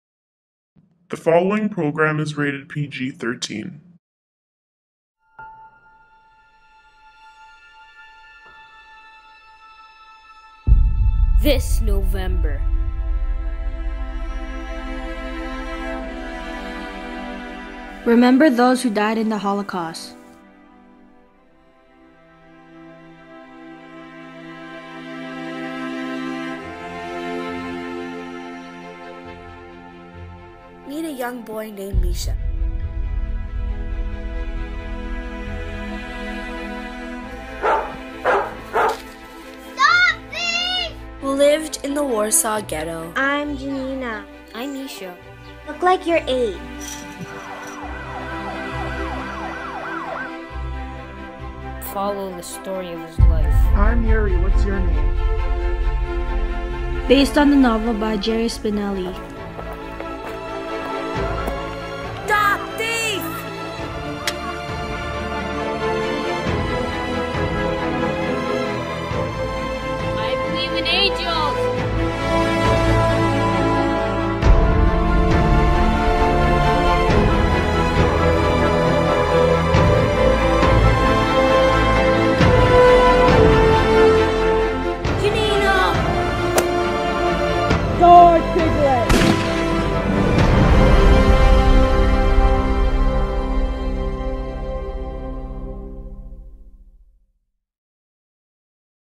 Milkweed Trailer by Jerry Spinelli
Trailer made for my grade 8 class.

Brandon Loureiro
Jeanine Placido
Mikayla Hamid
Gino Calabretta
Erandi Garcia

Enjoy Mrs. Galloro!